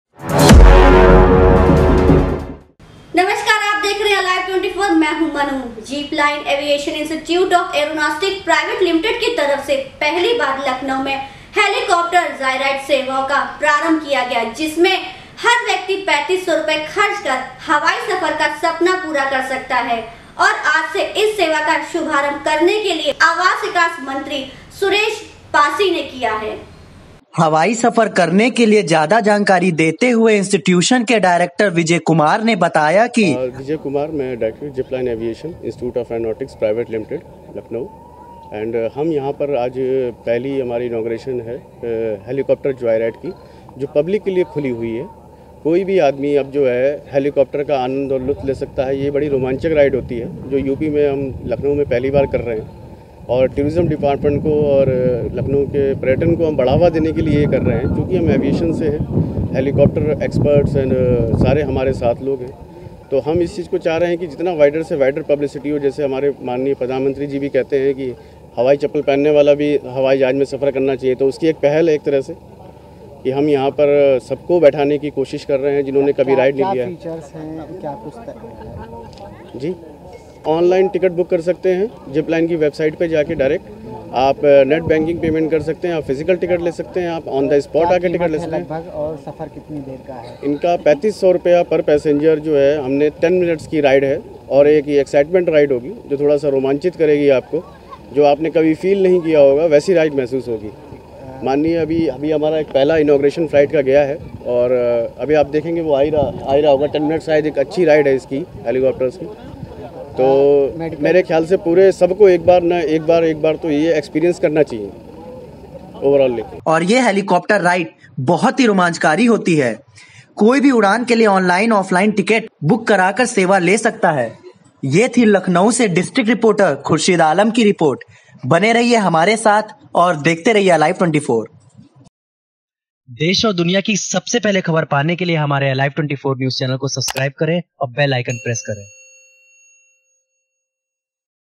अब लखनऊवासी ले सकेंगे हेलिकॉप्टर जायराइड सेवाओ का आनन्द ।
जीपलाइन ऐविएशन इंस्टियूशन ऑफ एयरोनॉटिक्स प्राईवेट लिमिटेड की तरफ से पहली बार लखनऊ में हेलिकॉप्टर जायराइड सेवाओ का प्रारम्भ किया गया जिसमें 35 सौ रुपये प्रति व्यक्ति खर्च कर हवाई सफर का सपना पूरा कर सकता है, आज से शुरू इस सेवा का सुभारम्भ करने के लिये आवास विकास मंत्री सुरेश पासी ने किया ।

वीओ - हवाई सफर करने के लिये ज्यादा जानकारी देते हुए इंस्टियूशन के डायरेक्टर विजय कुमार ने बताया आज उनकी हेलिकॉप्टर जायराइड की इनॉगरेशन है, इसमें हमारा उद्देश्य हर व्यक्ति का हवाई सफर सपना साकार करना है इसके लिए कोई भी व्यक्ति सिर्फ 35 सो रुपए खर्च कर पूरा कर सकता है और या हेलीकॉप्टर राइड बहुत ही रोमांचकारी होती है कोई भी उड़ान के लिए ऑनलाइन ऑफलाइन टिकट बुक करा कर सेवा ले सकता है ।
जिसके लिये पर log in कर टिकट बुक कराया जा सकता है ।

बाइट - विजय कुमार, जीपलाइन ऐविएशन इंस्टियूशन ।